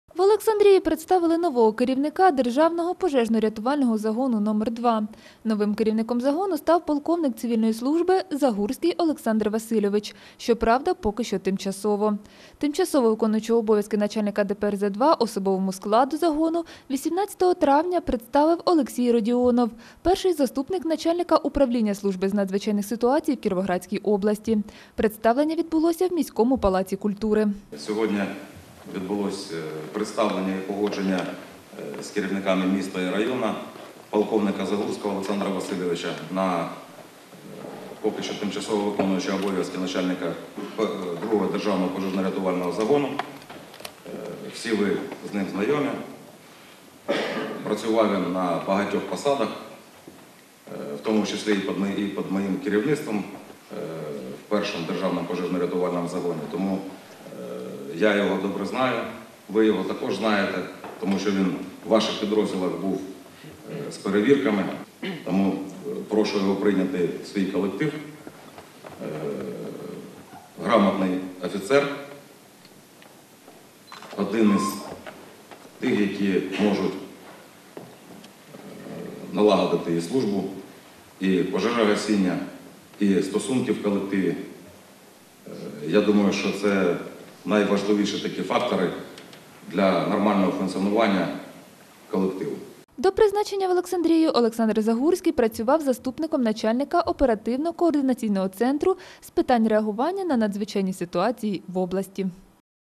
Новий керівник
В Олександрії представили нового керівника ДПРЗ-2